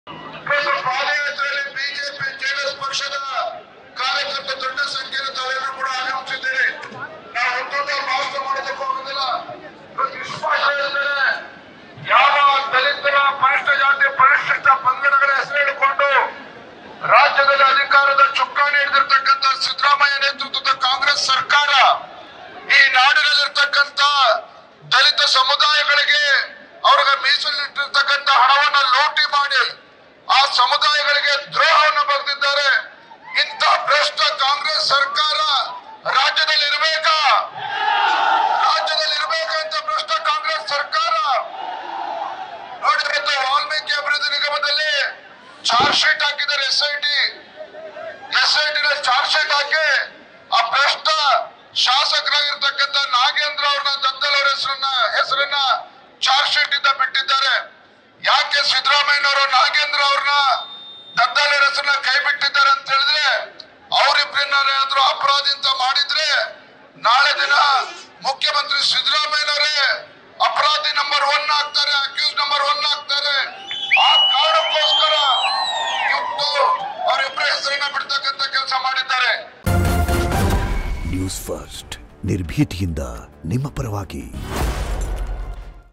BY Vijayendra: CM Siddaramaiah ಎಲ್ಲಿ ಅಪರಾಧಿ ನಂ1 ಆಗ್ಬಿಡ್ತಿನೋ ಅಂತ ಇಂಥ ಕೆಲಸ ಮಾಡಿದಾರೆ| BJP JDS Padayatra
BY Vijayendra : CM Siddaramaiah ಎಲ್ಲಿ ಅಪರಾಧಿ ನಂ.1 ಆಗ್ಬಿಡ್ತಿನೋ ಅಂತ ಇಂಥ ಕೆಲಸ ಮಾಡಿದಾರೆ | BJP JDS Padayatra | @newsfirstkannada